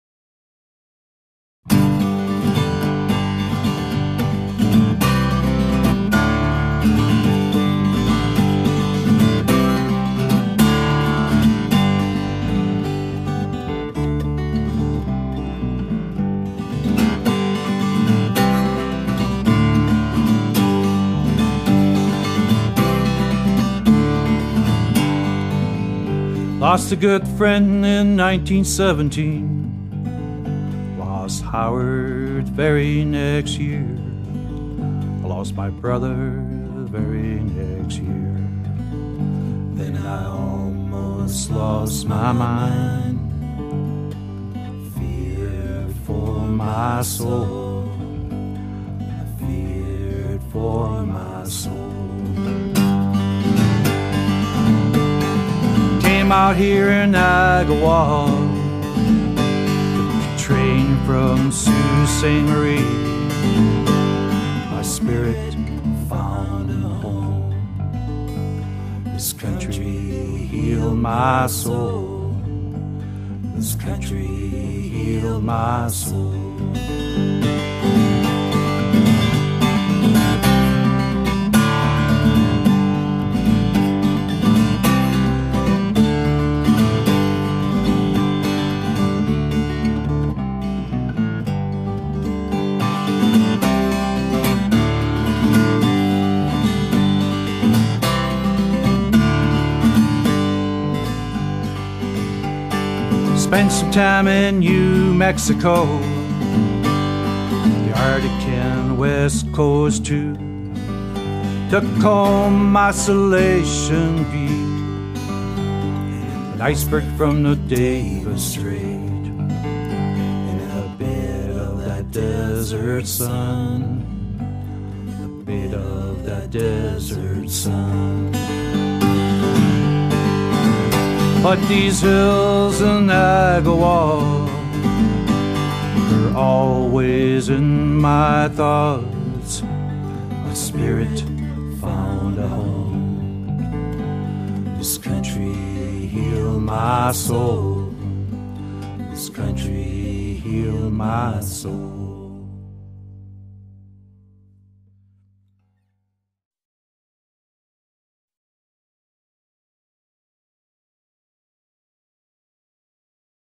A Tribute to Group of Seven artist Lawren Harris
The song on this video is dedicated to the memory of Lawren Harris. He came to Agawa Canyon in the spring of 1919 with a friend ... Harris felt this country healed his soul and renewed his spirit!